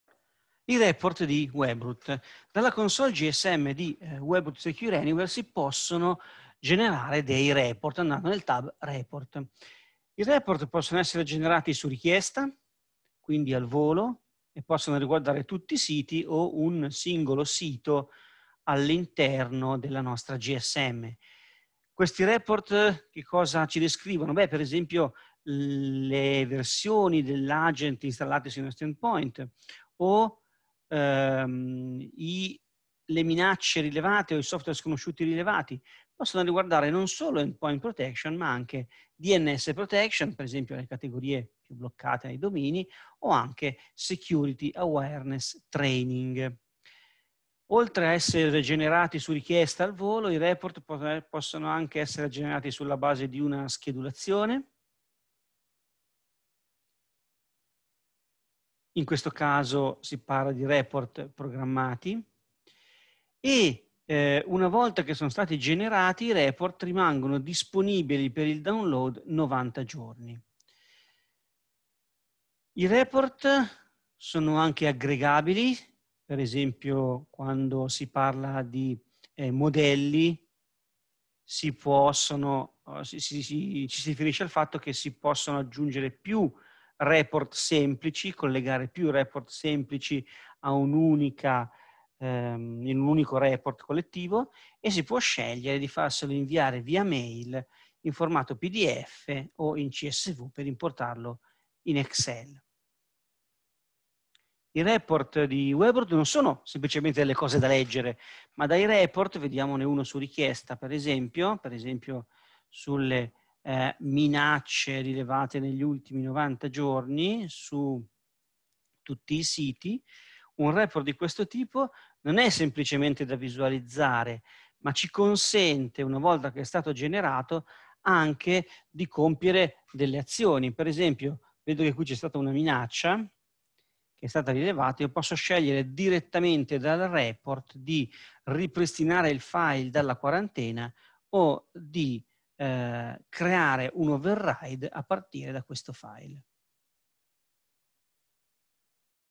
Webroot: report
Scopri cosa sono, dove si trovano e a cosa servono i report di Webroot.